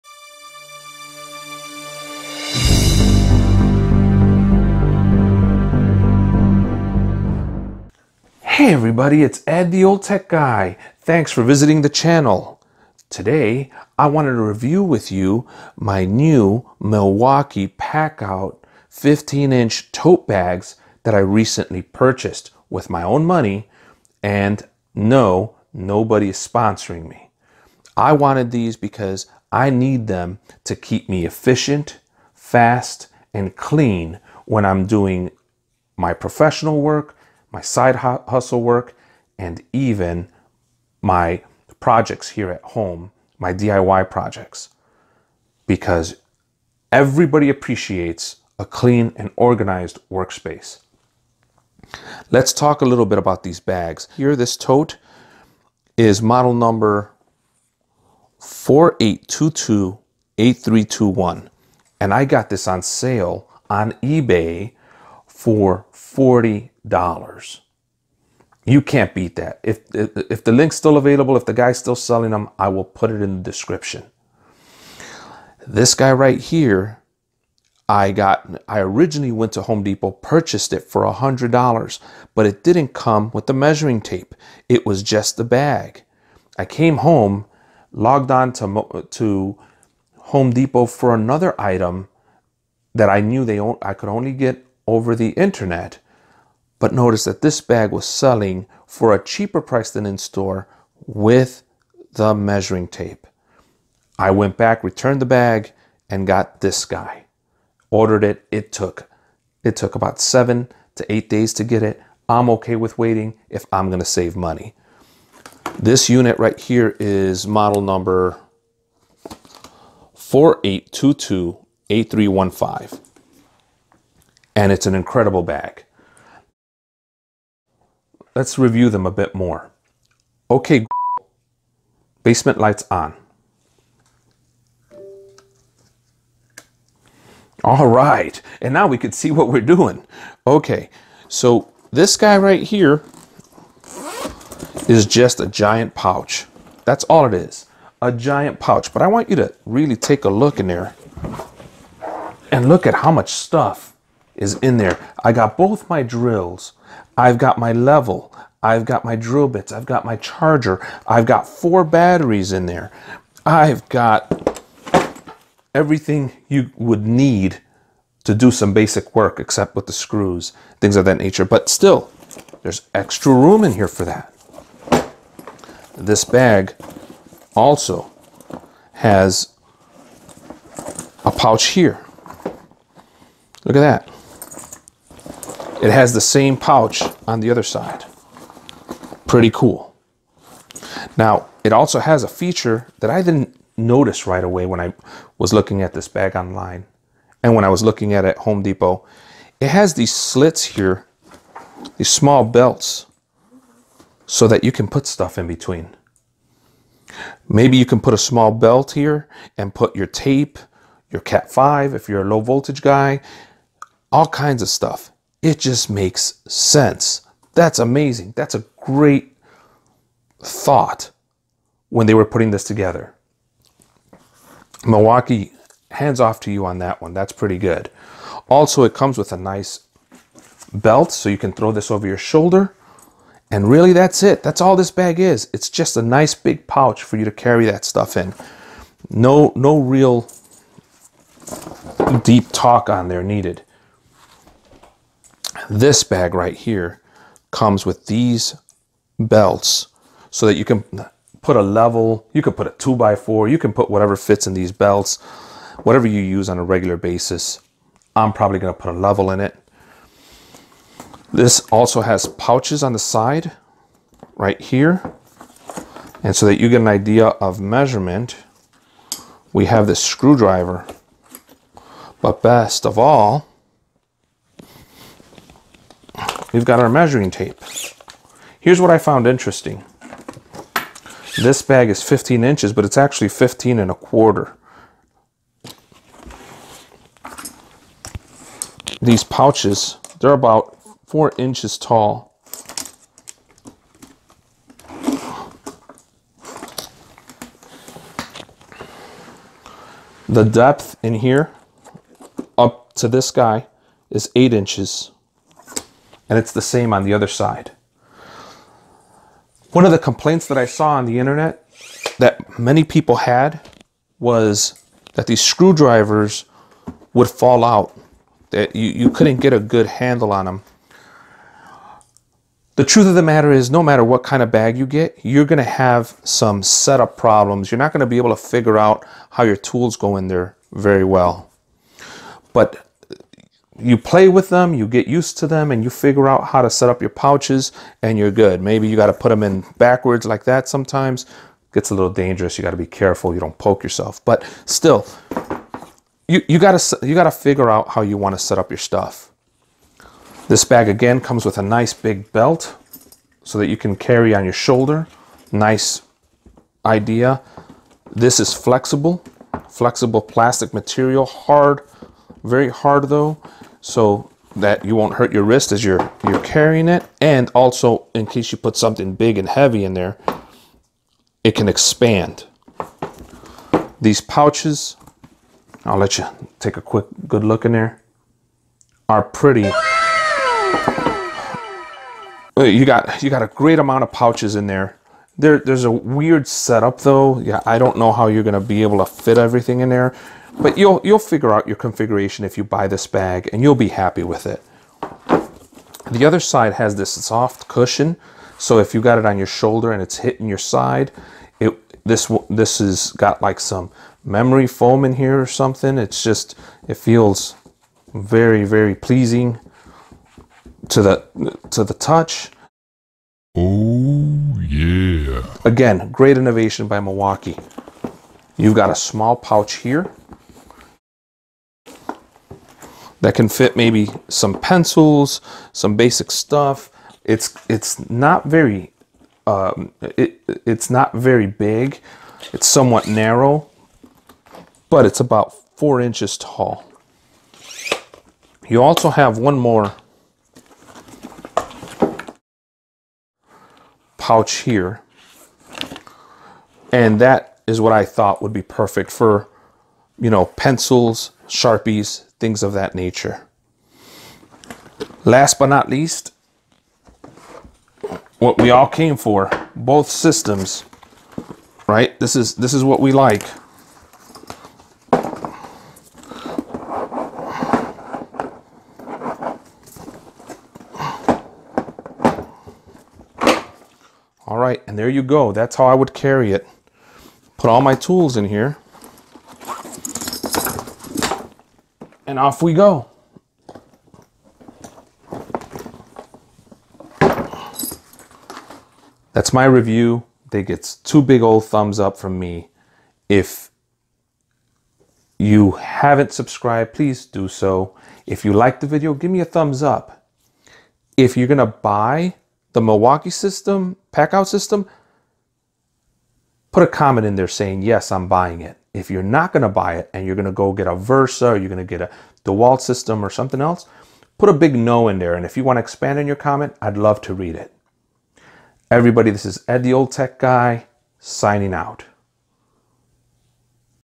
Deal Alert! The Best Milwaukee Tool bags! Sorry Dewalt!
Description:

edtheoldtechguy reviewes the 15" Milwaukee tool bags / storage totes from the packout system. You should rush to get these bags on sale now.

15 in. PACKOUT Tote with 25 ft. Stud Tape Measure

Impact resistant molded base attaches to other PACKOUT products
Includes 25 ft. STUD Tape Measure
Part of the PACKOUT Modular Storage System

Run! Run! Run! the Ebay listing is still available as of 12/22/2019!

Milwaukee packout Tool Bag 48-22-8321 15” Ballistic M18 Storage Solution
Ebay

Milwaukee packout Tool Bag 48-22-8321

15” Ballistic M18 Storage Solution. Condition is New.